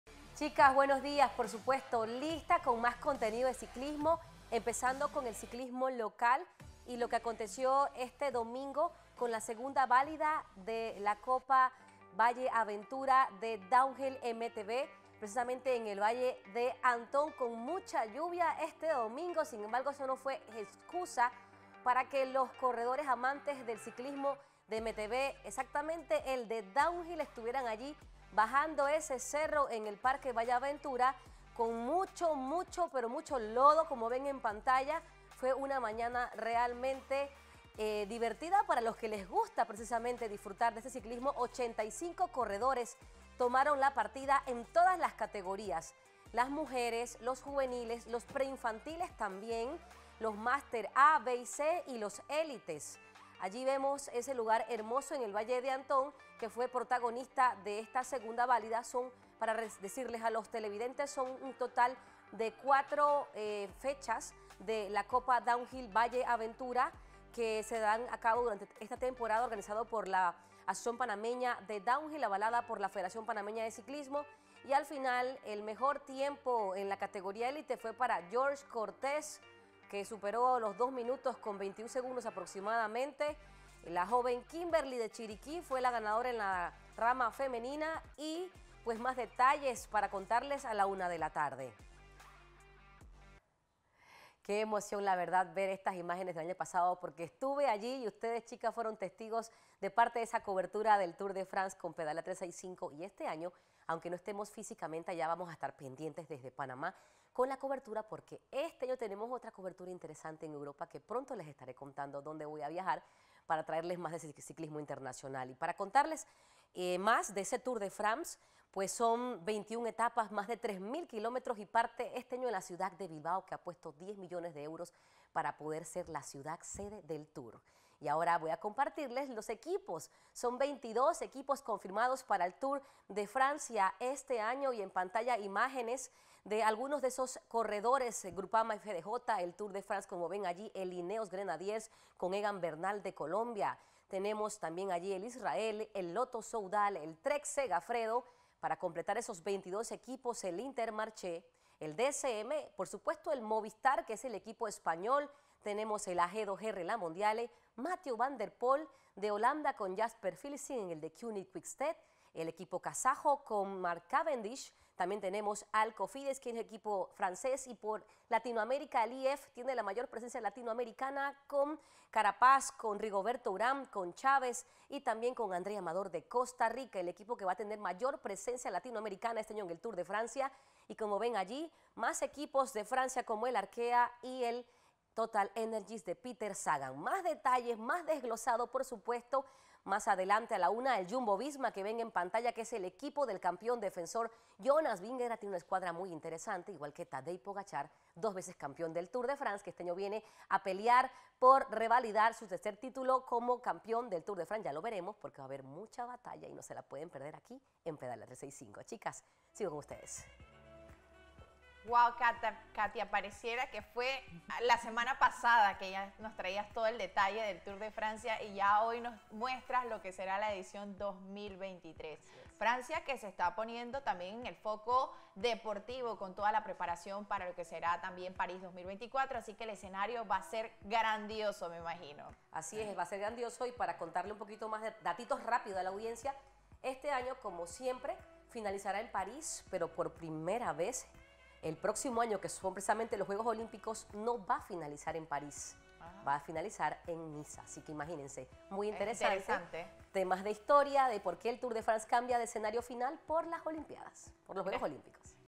Downhill Series en Panamá y los 22 equipos del Tour de Francia 2023
Te traemos la segunda fecha de la Copa Downhill Valle Aventura y el listado de los equipos que participarán en la Edición 110 del Tour de Francia.